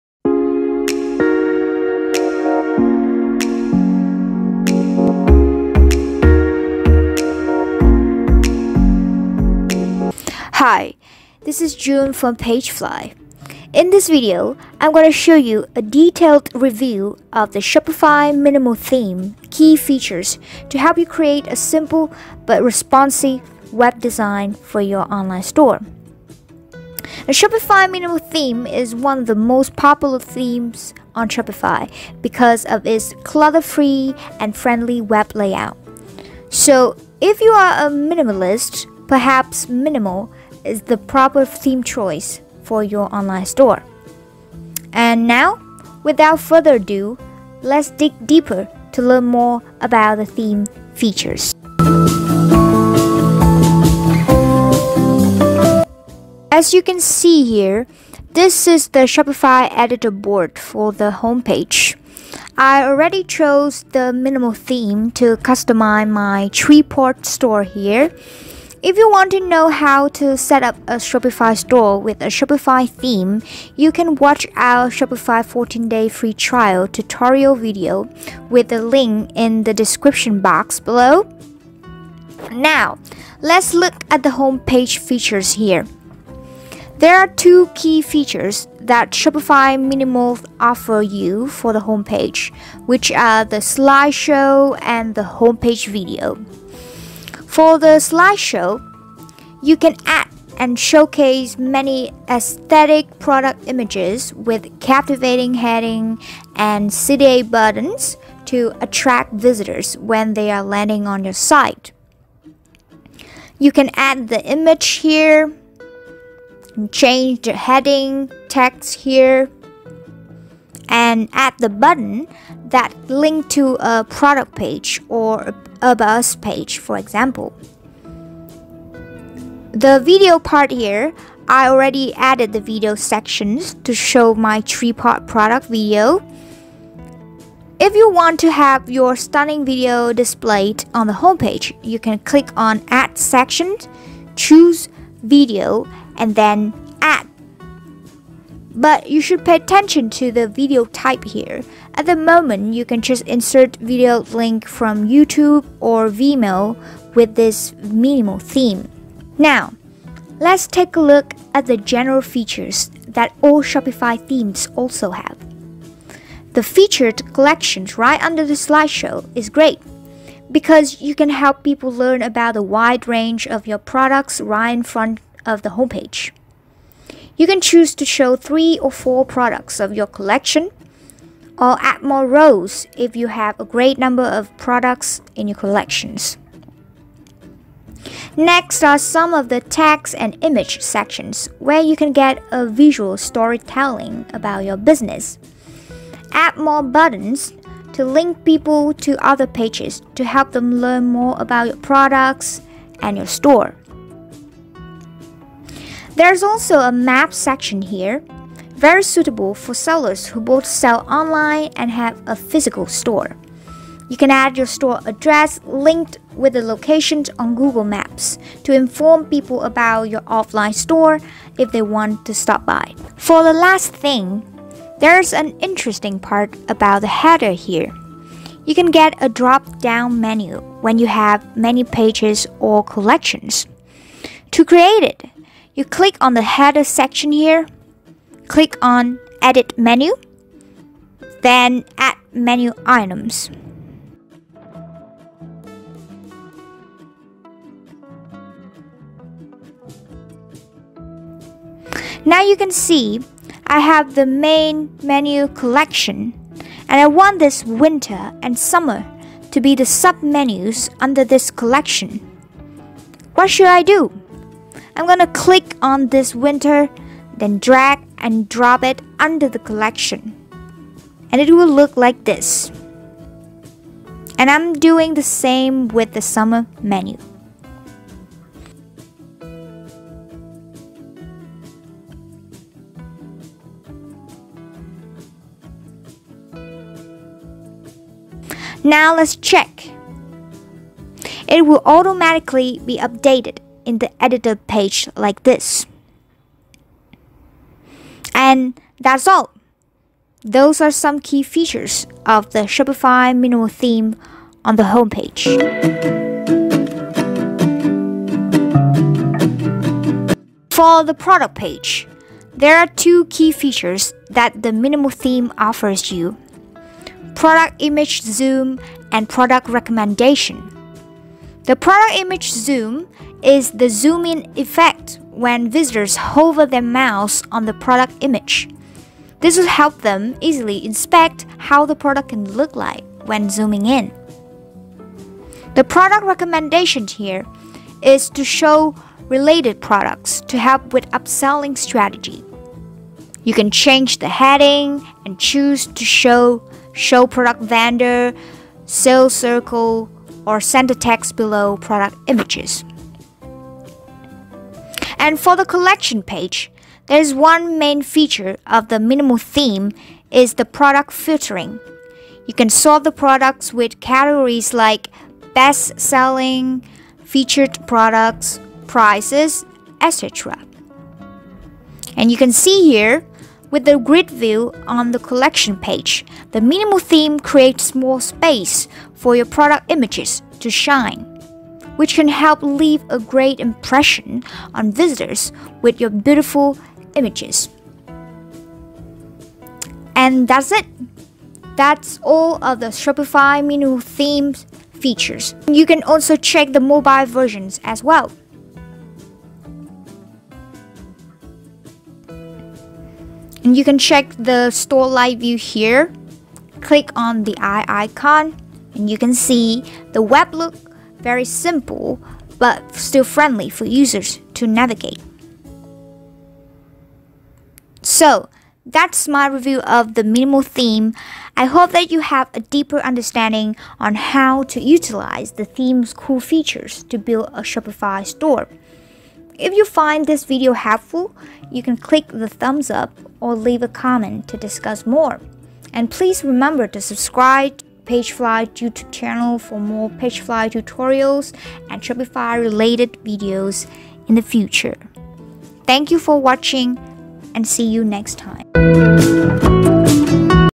Shopify Minimal Theme: In-depth Features Review and Tutorial
Shopify Minimal Theme: a suitable choice for merchants who want a simple but friendly web layout. In this video, we are going to give you a deep-dive review of the theme key features and a brief tutorial on how to build a store with the Shopify Minimal Theme. 👇Click on “SHOW MORE" to see the outline👇

👉 Build Your Own Custom Shopify Store - For FREE - with 60+ Templates and elements for Conversion

⭐Outline⭐
00:00 - 00:51 Intro
00:52 - 05:04 Shopify Minimal theme Homepage key features
05:05 - 07:30 Product and Collection pages key features